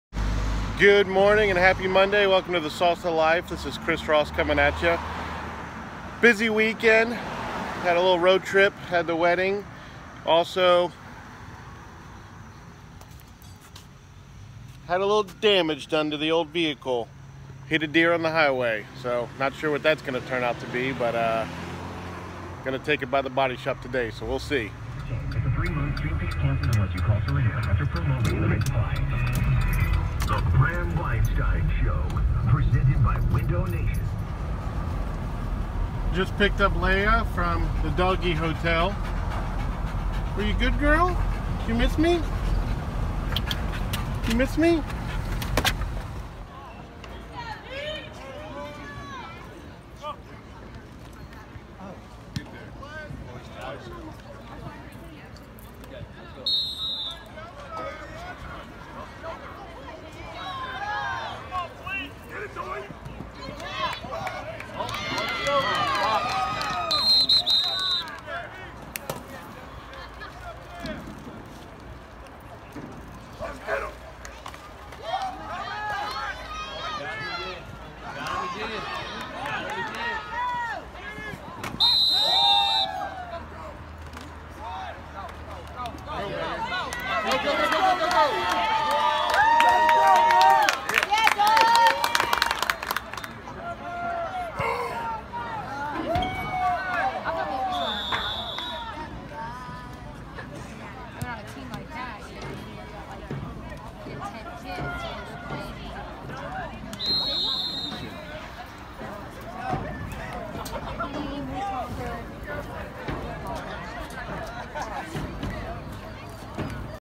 Sauce of Life - Coming back after a big weekend Vlog 22
Coming back after a big weekend and some more Bulldog Football.
SauceofLife Vlog #22